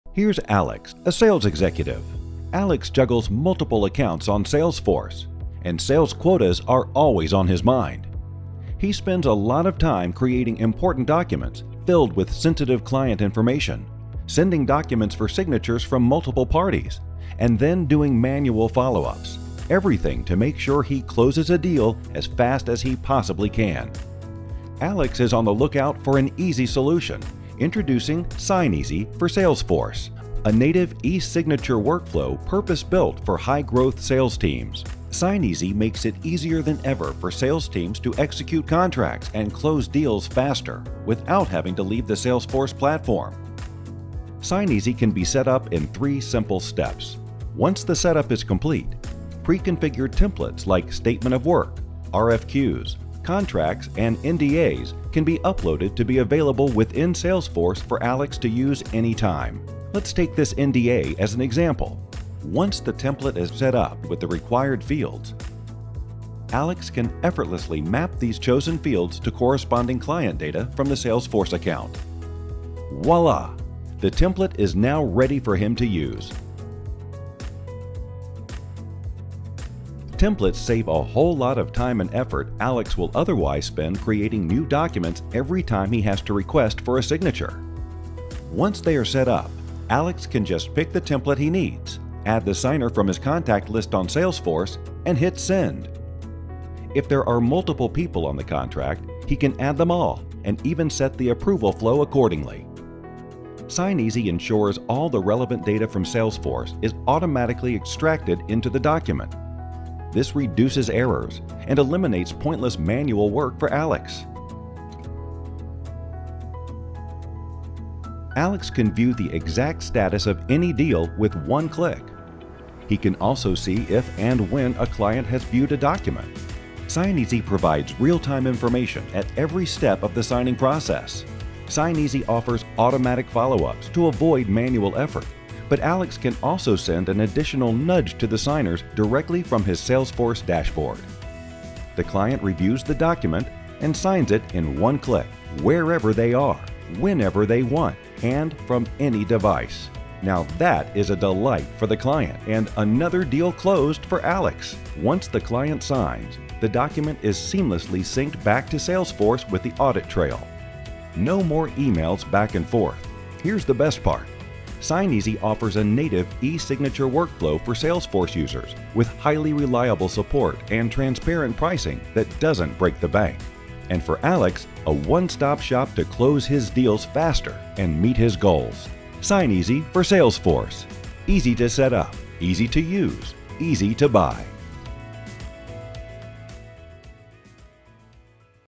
Signeasy for Salesforce | Easy eSignatures for high-growth sales teams
Signeasy for Salesforce is a native eSignature workflow purpose-built for high-growth sales teams.
Close deals faster with electronic signature workflows within Salesforce. Prepare, send, track and eSign all your contracts and agreements with ease.